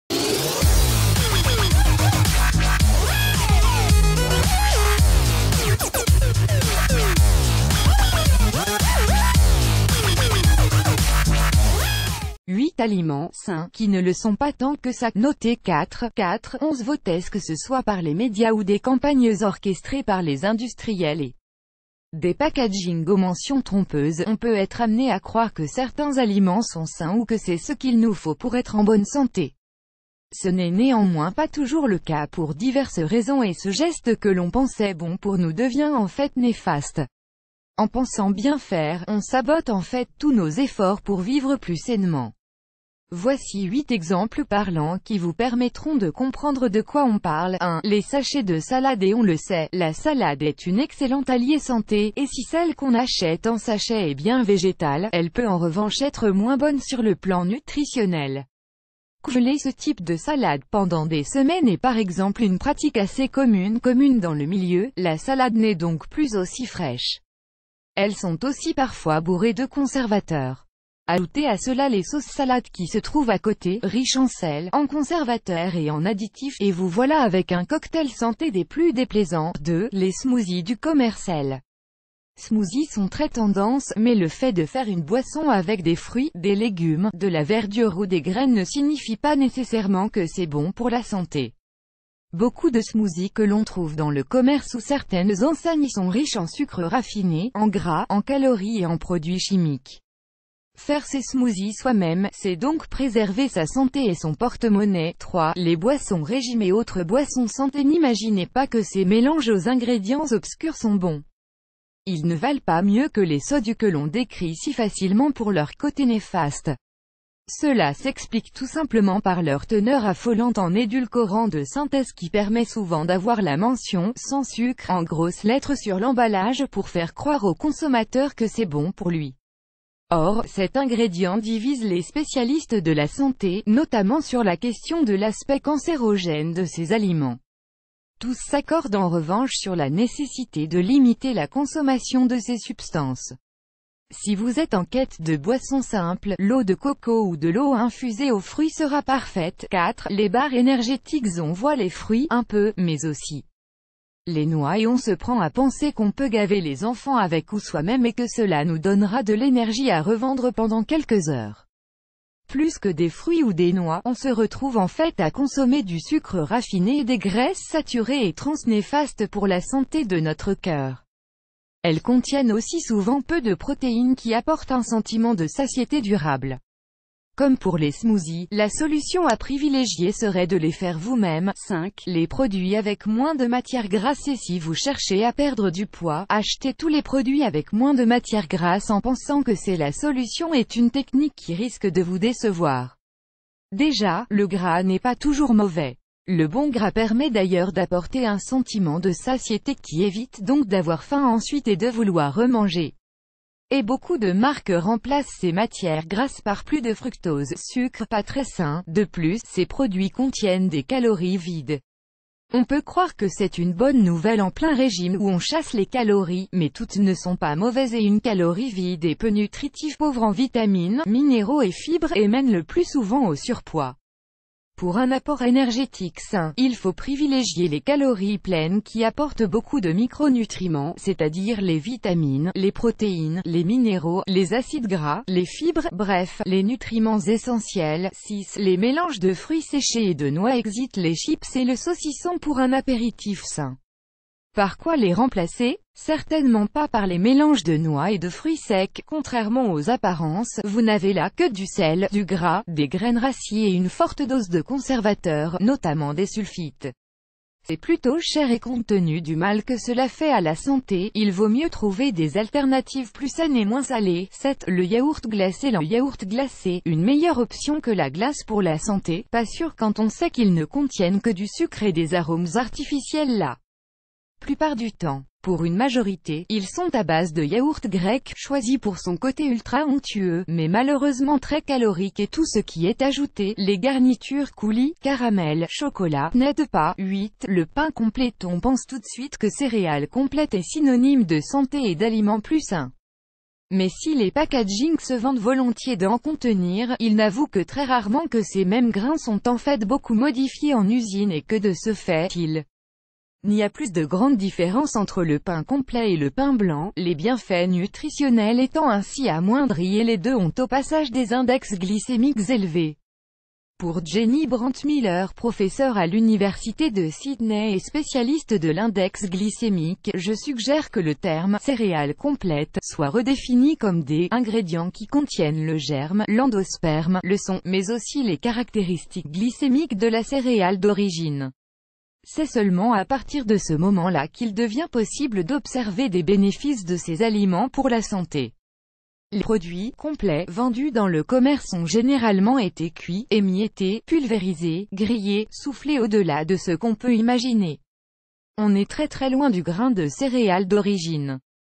8 aliments « sains » qui ne le sont pas tant que ça !
8 aliments « sains » qui ne le sont pas tant que ça !
8 aliments « sains » qui ne le sont pas tant que ça !noté 4.4 - 11 votesQue ce soit par les médias ou des campagnes orchestrées par les industriels et des packagings aux mentions trompeuses, on peut être amenés à croire que certains aliments sont sains ou que c’est ce qu’il nous faut pour être en bo...